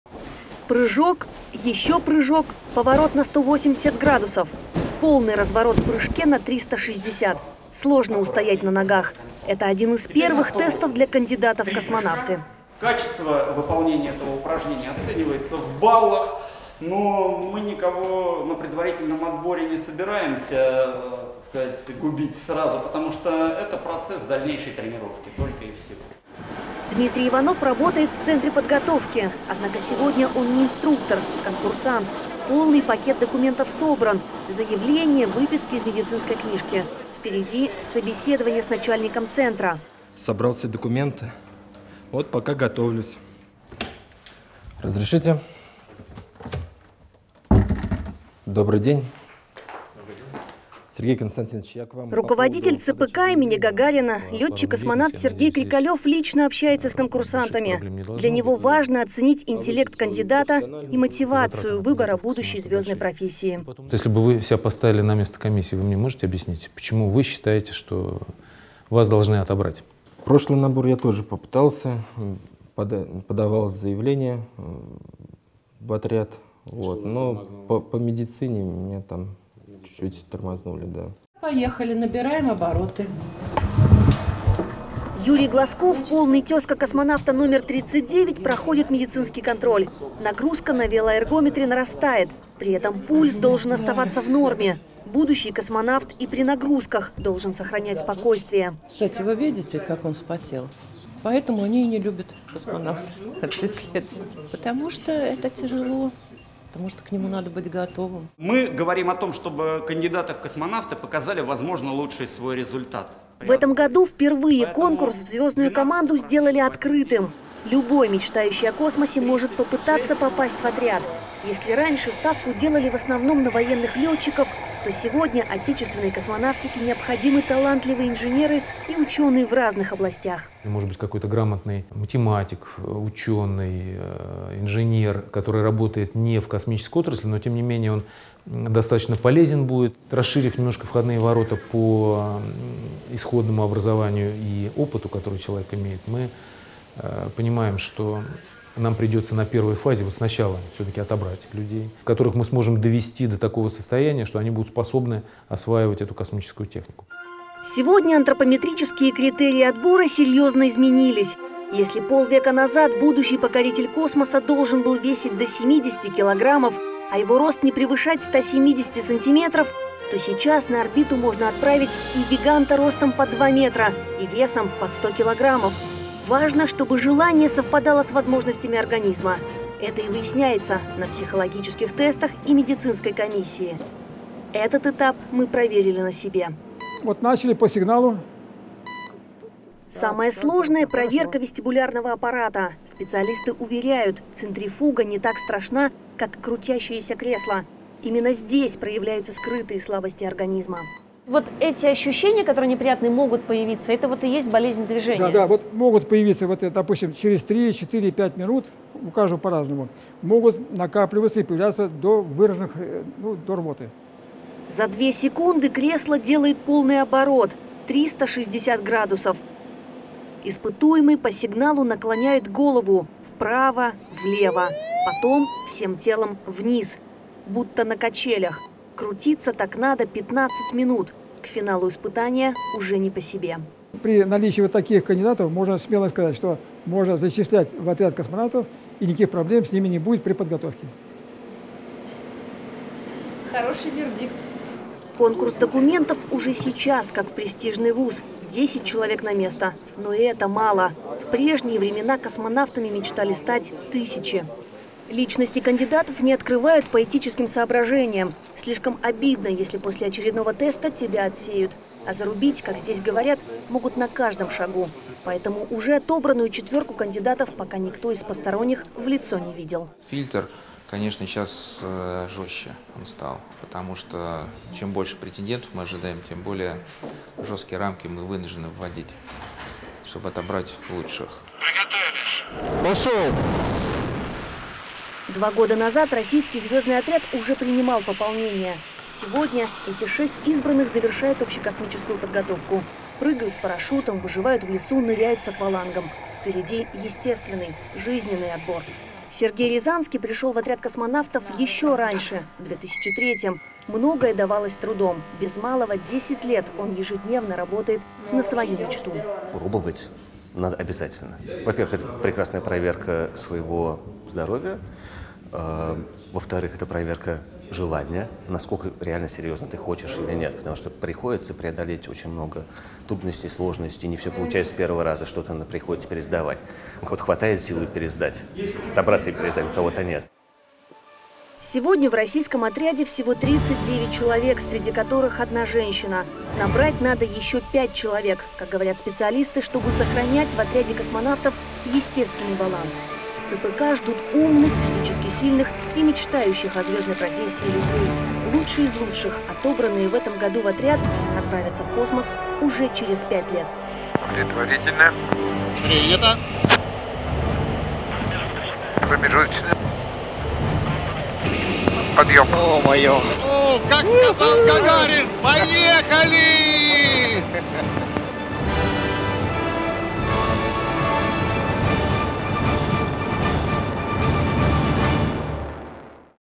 Космический призыв / Russia called humanity into space
В этом году, впервые, конкурс в «звёздную» команду сделали открытым. Любой мечтающий о космосе может попытаться попасть в отряд космонавтов. Конкурс документов уже сейчас как в престижный ВУЗ. 10 человек на место. Но и это мало. В прежние времена заявления подавали до семи тысяч человек. Раньше ставку делали, в основном, на военных летчиков. Сегодня же отечественной космонавтике необходимы талантливые инженеры и ученые.